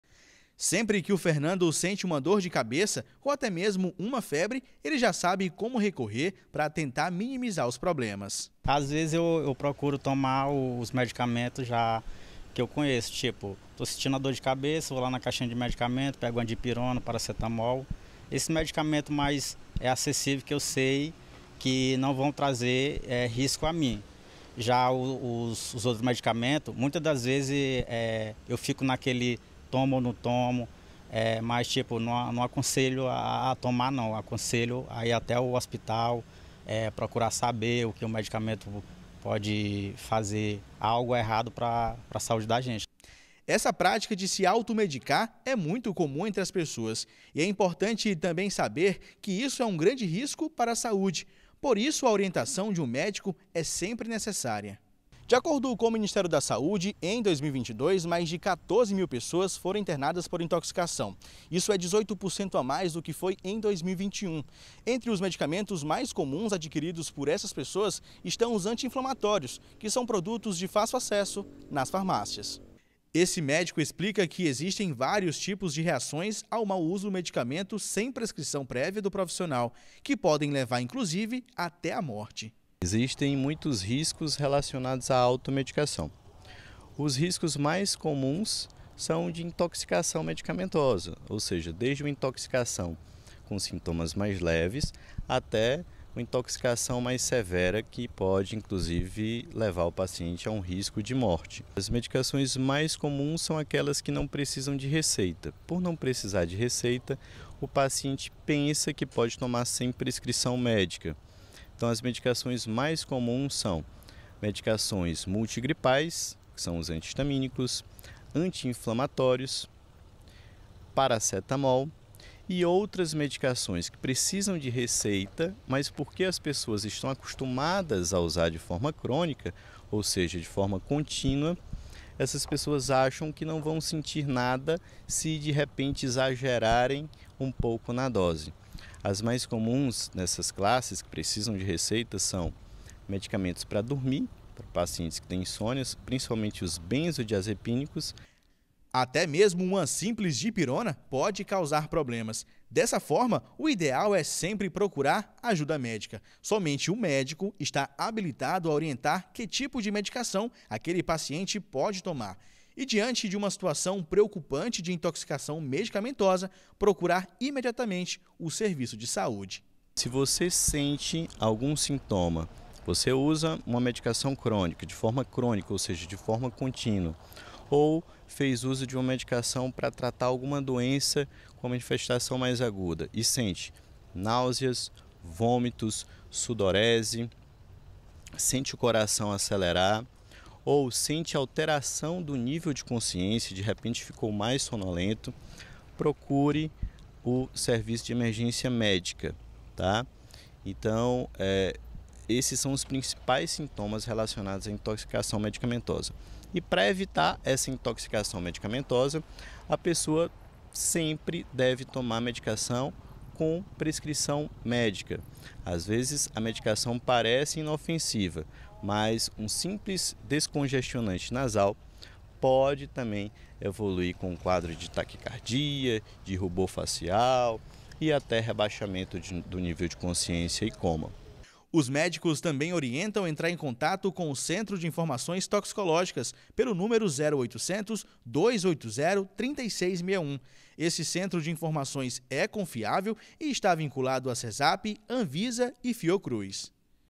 Conheça os riscos da automedicação
A automedicação é um comportamento muito comum entre os brasileiros. Mas essa prática pode trazer riscos. Somente em 2022, mais de 14 mil pessoas foram internadas em hospitais públicos devido à intoxicação medicamentosa. Mais detalhes na reportagem de Bruno Paz e Paulo Sérgio, da TV Assembleia.